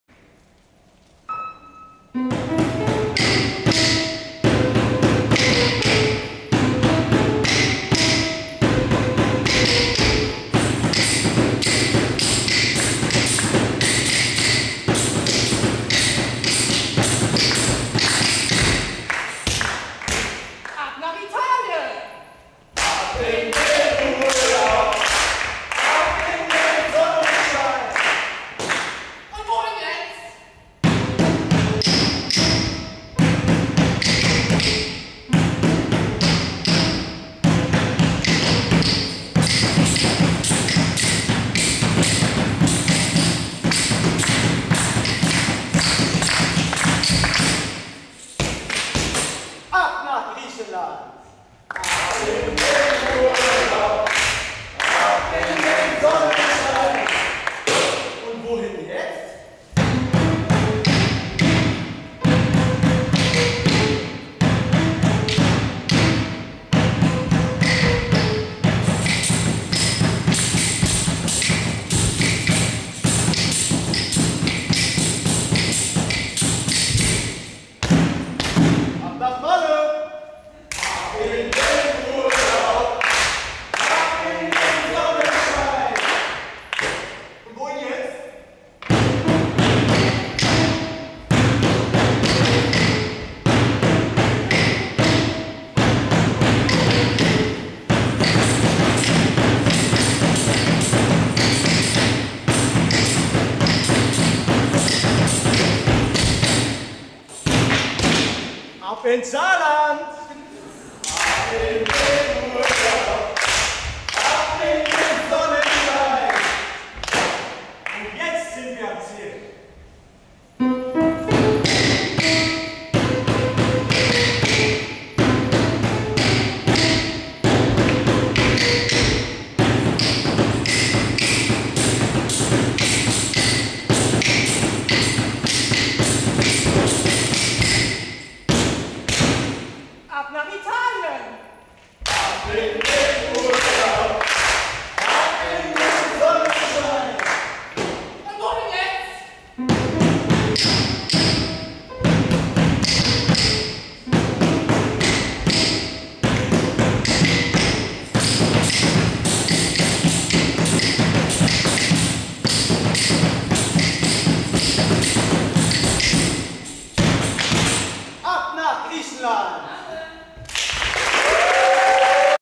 Die Urlaubspiloten (1)
An die TV-Werbung von "Ab-in-den-Urlaub.de" angelent, nehmen die Tänzer die Zuschauer mit auf eine außergewöhnlich, abwechslungsreiche Reise quer durch Europa und versetzen sie so in Urlaubsstimmung.